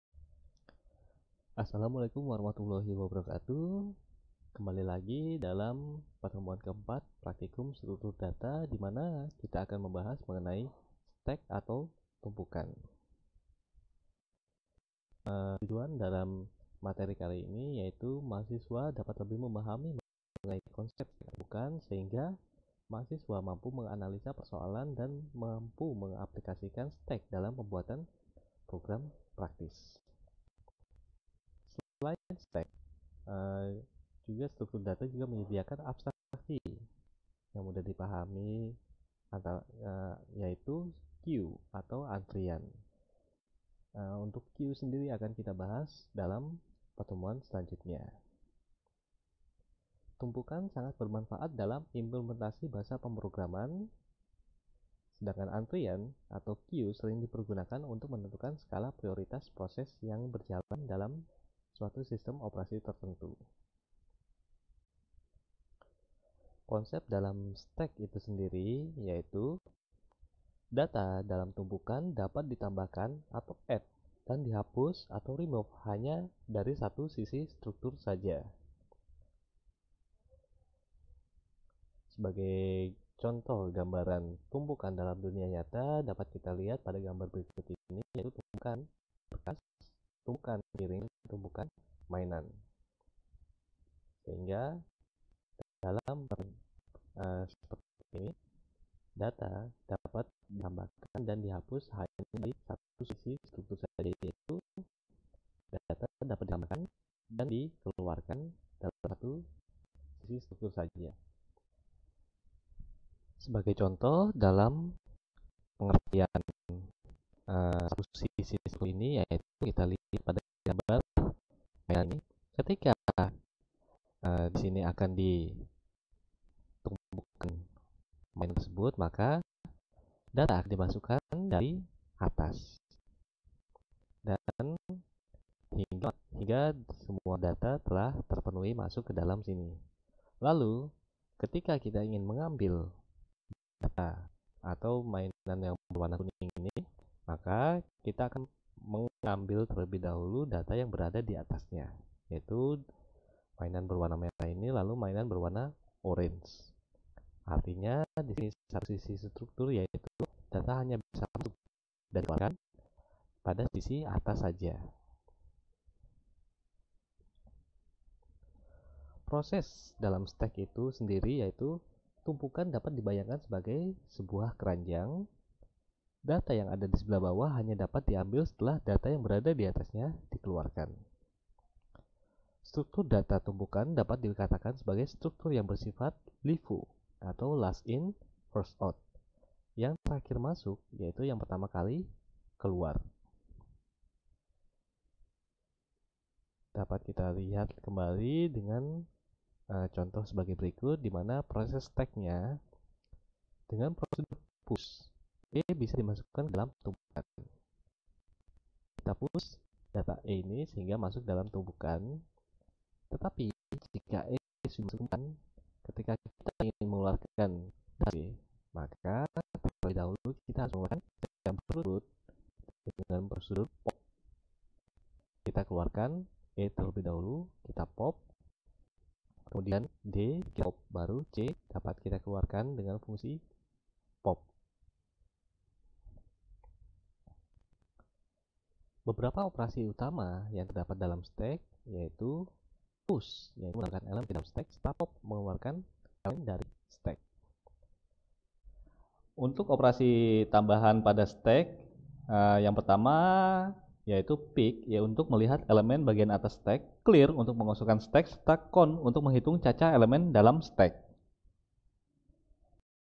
Struktur Data - Stack Teori
Dalam materi ini membahas mengenai stack / tumpukan